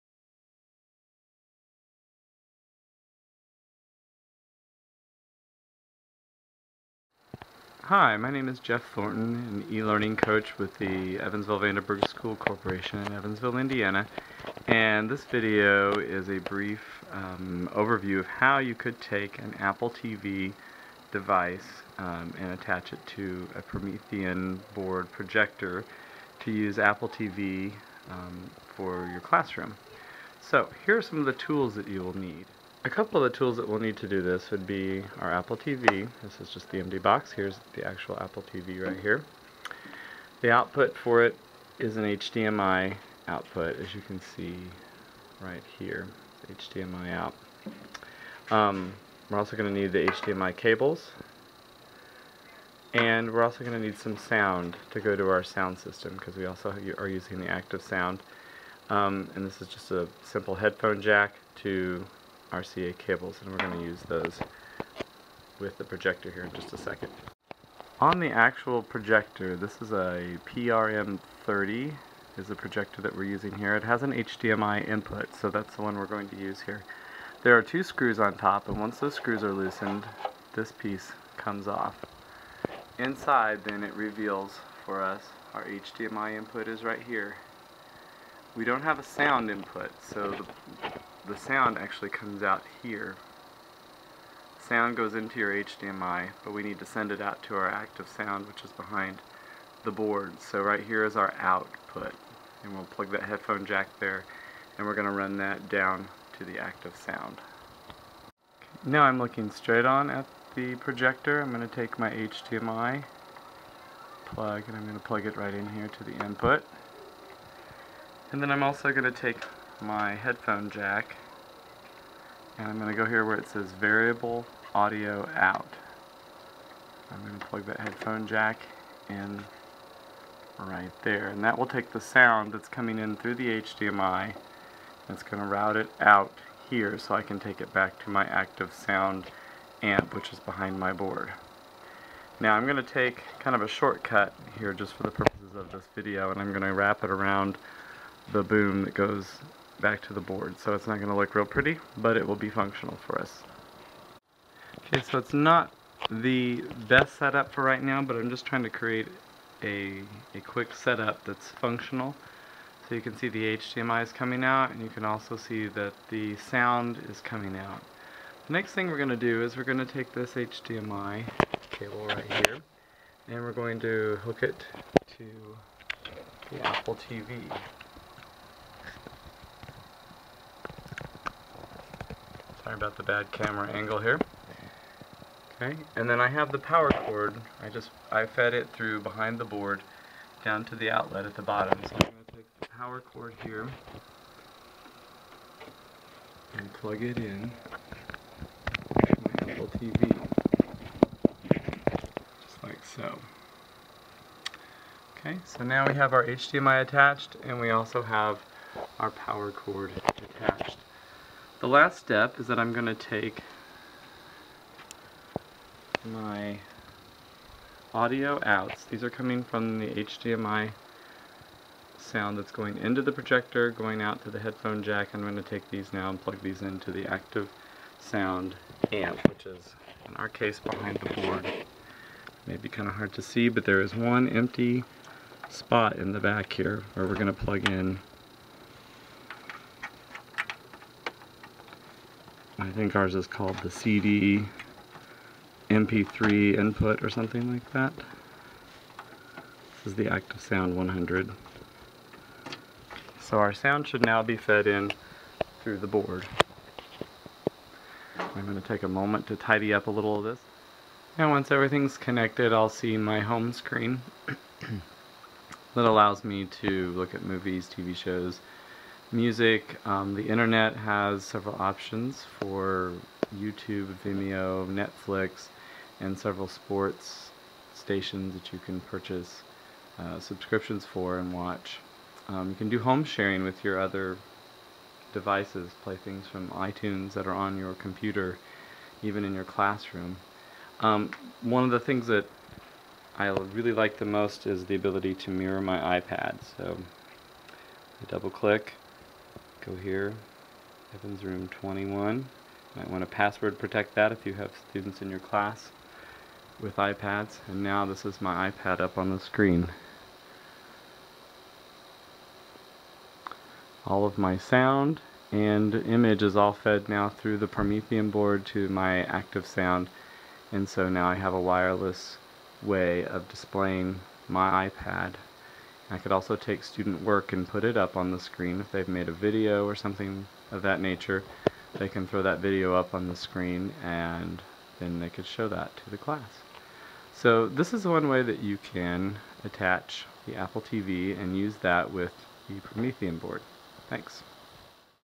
Apple TV for Promethean Board
This tutorial explains how to add an Apple TV device to a Promethean Board.